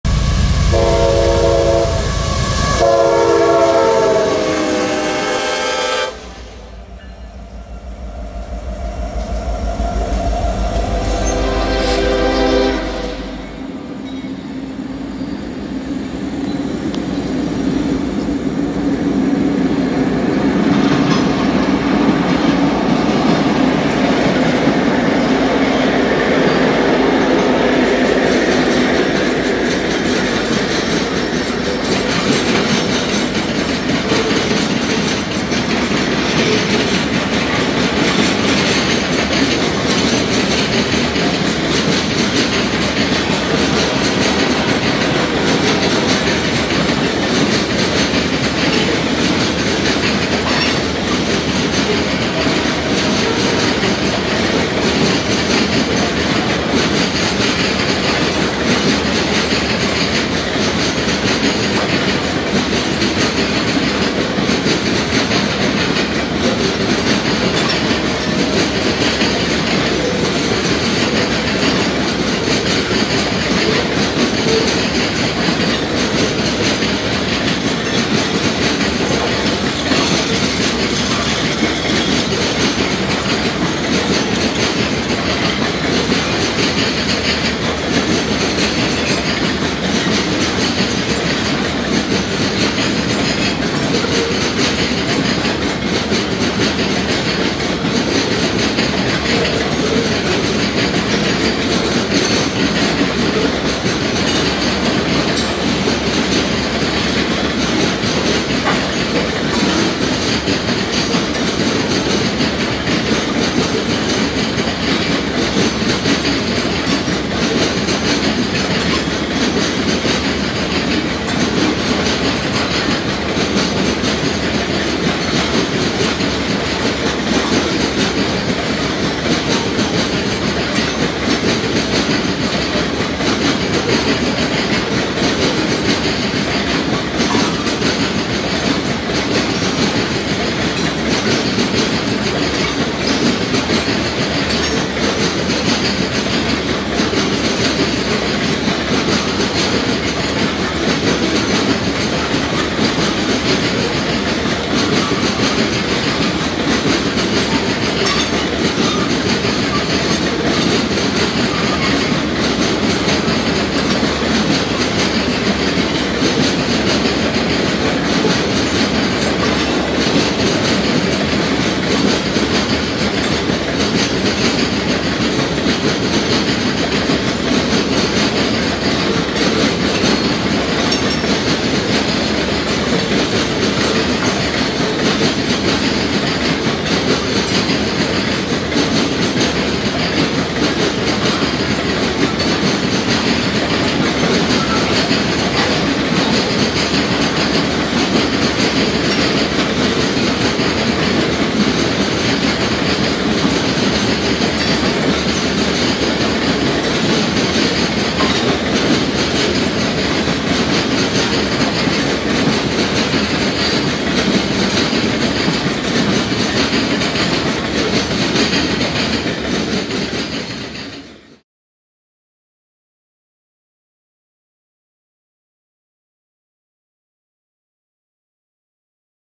CSX Manifest Train W/BNSF Cordele Ga
CSX W/BNSF Power NB on the Fitzgerald Sub. with Covered Hoppers.

CSX 5354 Lead
BNSF 7435
BNSF 9128